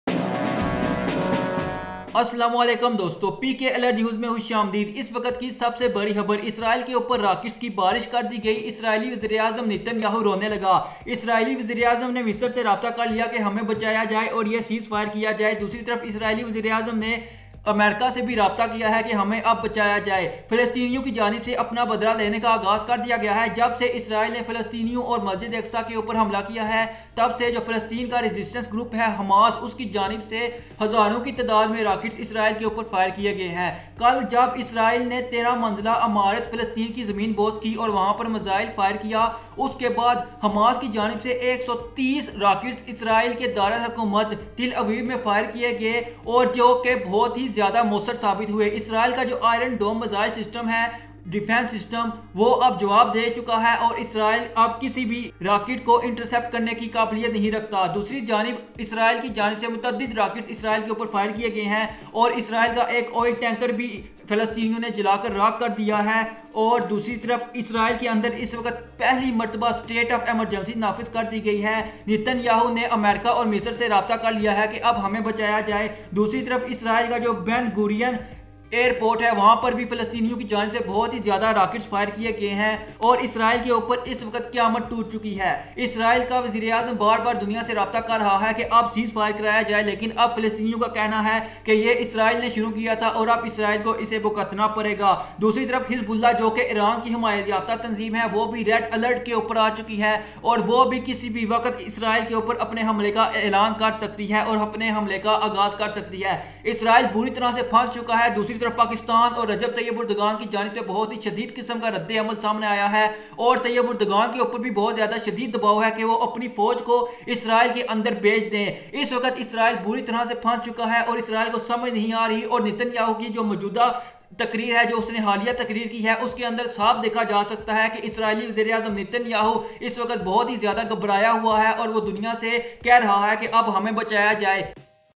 Huge Rocket Barrage Fire on Israel From Gaza || Israel Attack Palestine || Breaking News
Almost 200 rockets barrage fire from Palestine towards Israel. Israel Also attack on Palestine and killed innocent Palestinians. Pakistan and international media reporting about this.